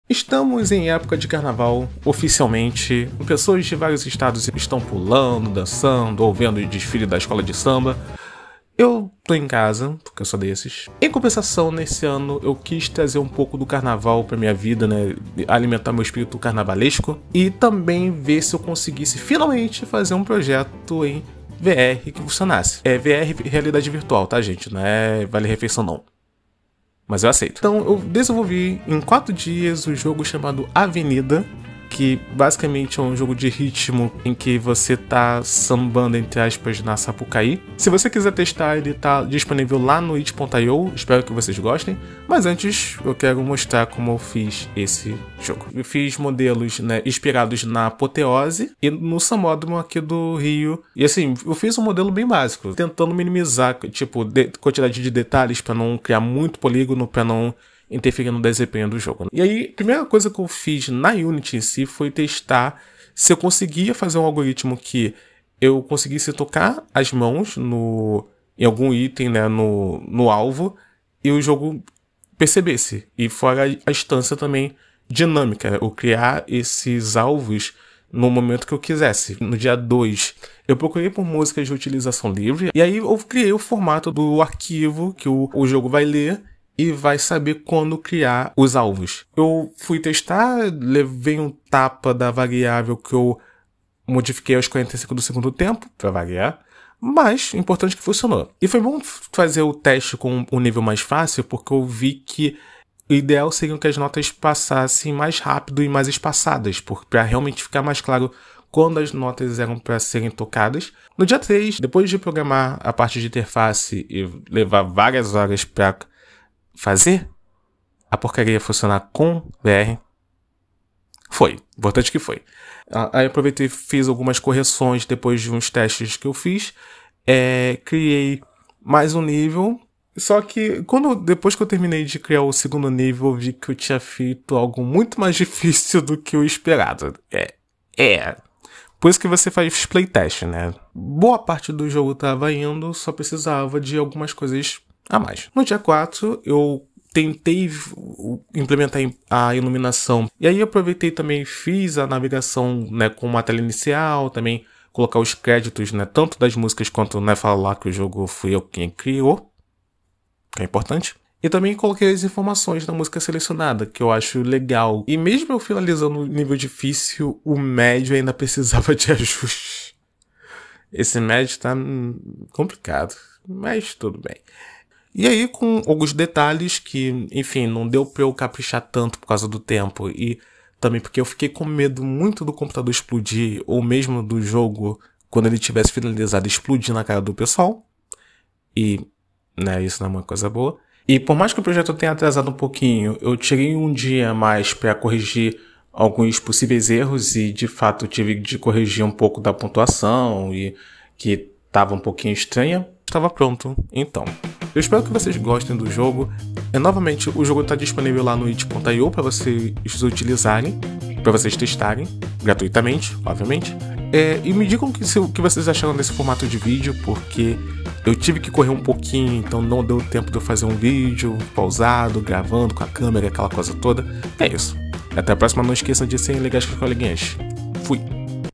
Fiz um jogo VR de Carnaval - Avenida (feito em Unity)
Há alguns dias, eu tenho pensado sobre fazer um projeto envolvendo carnaval e VR e tentei desenvolver esse jogo de ritmo chamado Avenida. Por causa do tempo, não deu para adicionar muitos detalhes (eu até queria colocar uma mini plateia reagindo à sua performance e colocar texturas simples ou animações para o céu, mas o importante é que eu posso dizer que já trabalhei com jogos em VR.